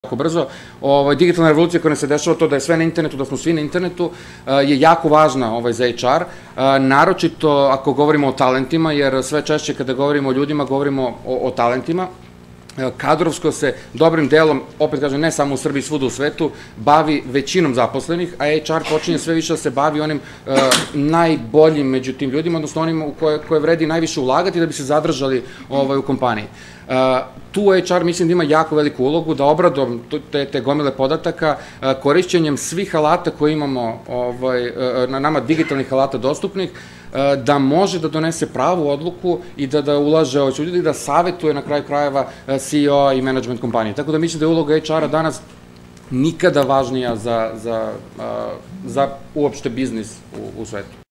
Nikola Jovanović - Sve češće kad govorimo o ljudima, govorimo o talentima
HR Srbija 2015